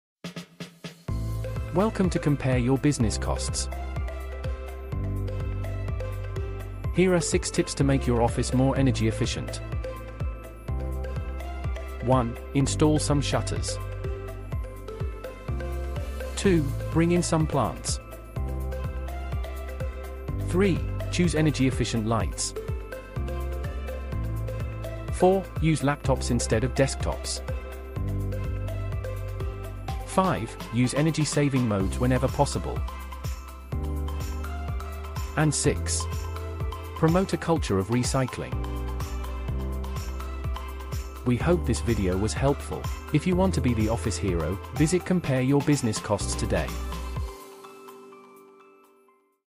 6 Proven Tips to Boost Office Energy Efficiency
Transform your office into an energy-saving powerhouse with these 6 practical tips! 🌿💼 We've gathered expert insights on lighting, appliances, and more to help you reduce energy costs and environmental impact. Join us to create a greener, more efficient workspace! 🏢💡